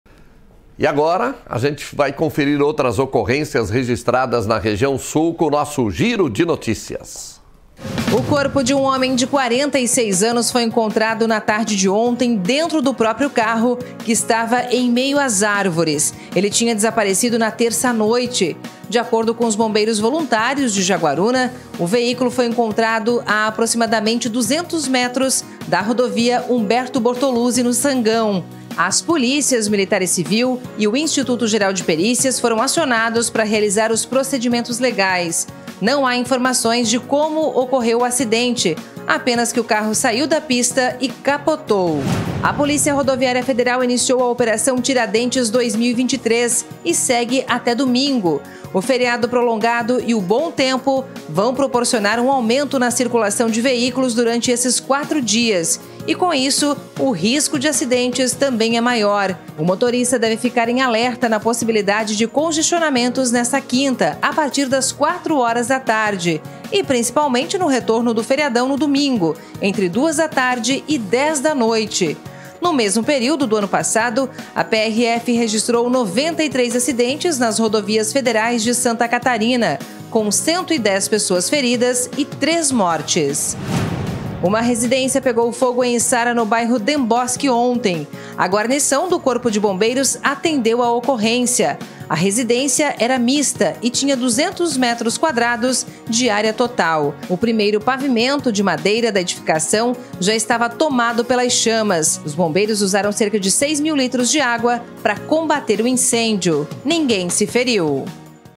Giro de notícias: homem é encontrado morto dentro de veículo em Sangão
O homem estava desaparecido desde terça-feira e foi encontrado morto em seu veículo. Esse é um dos destaques do Giro de Notícias do BG.